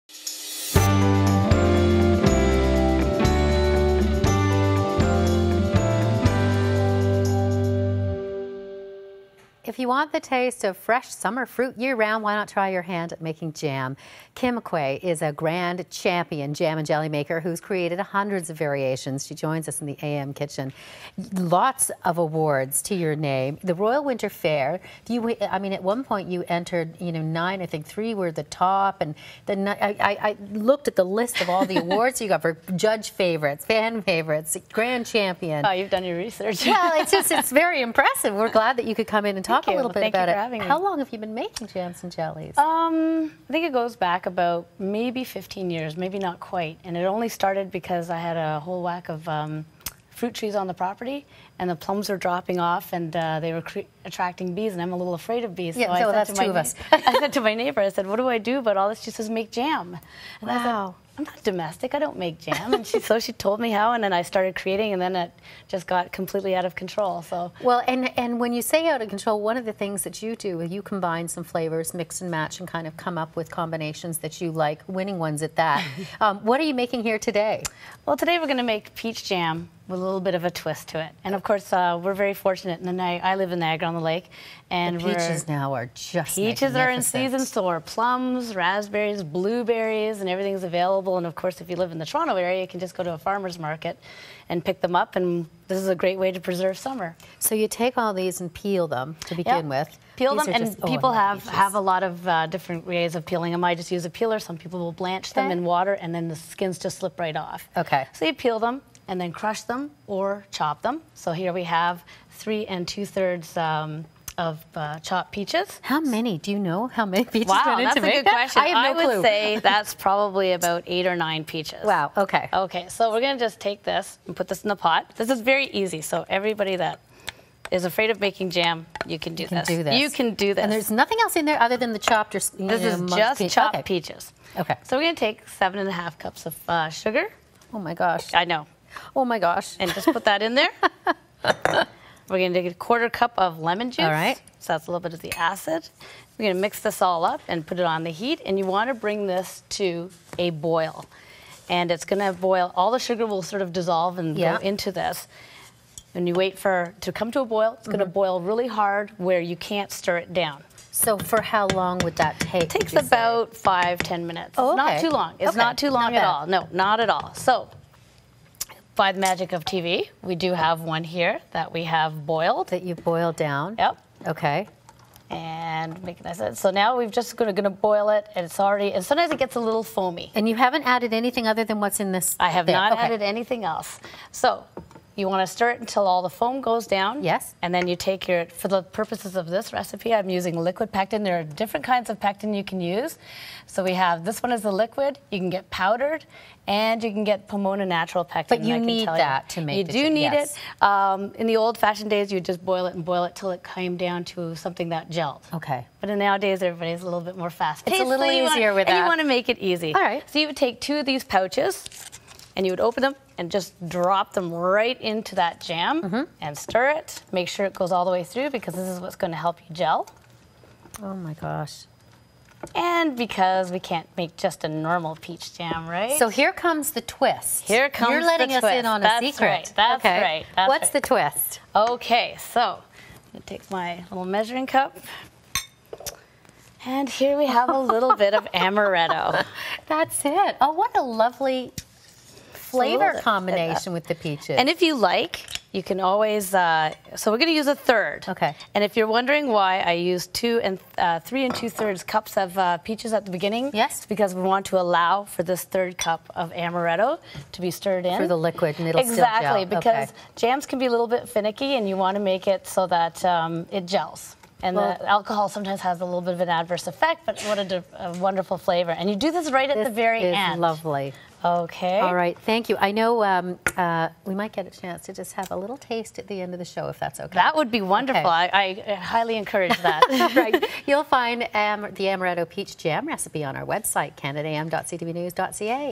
Jam with a twist: Amaretto Peach Jam
Award-winning jam and jelly maker Kim McQuhae makes amaretto peach jam.